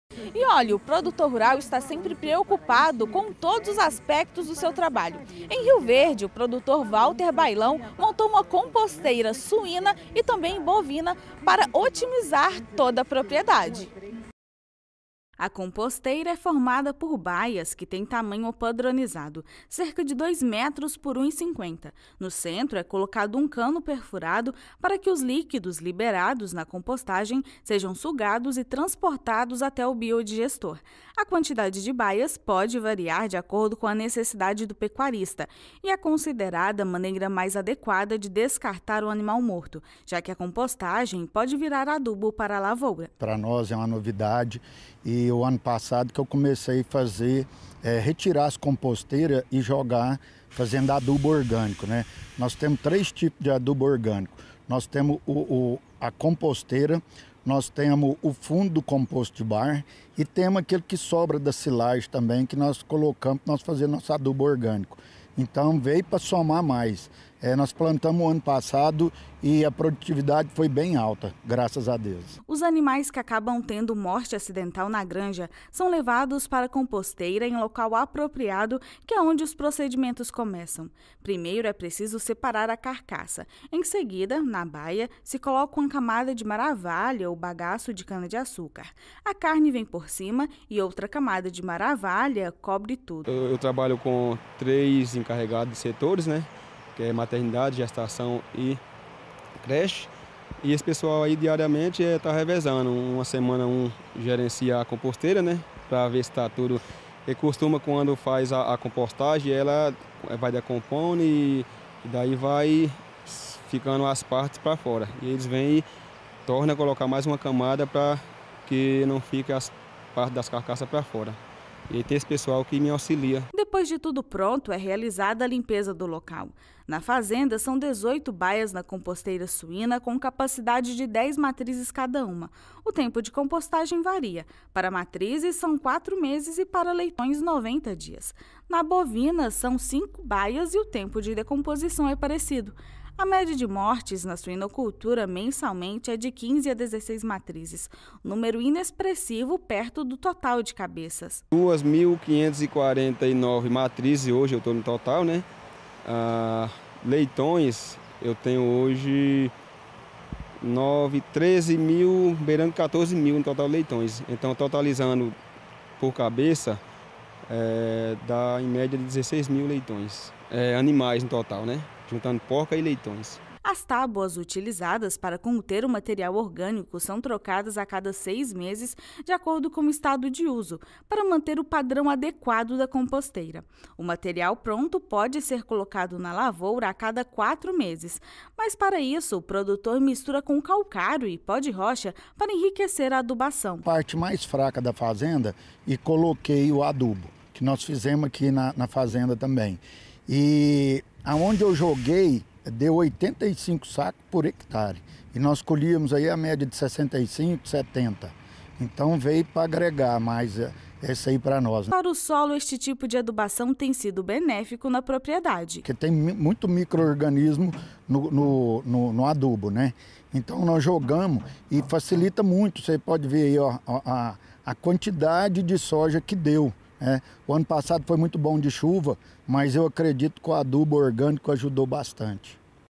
Composteira suína e bovina
Na atividade pecuária o produtor rural se preocupa em todos os aspectos da do trabalho, em Rio Verde Walter Baylão desenvolveu a composteira com dejetos suínos e bovinos o que auxilia na sustentabilidade da fazenda.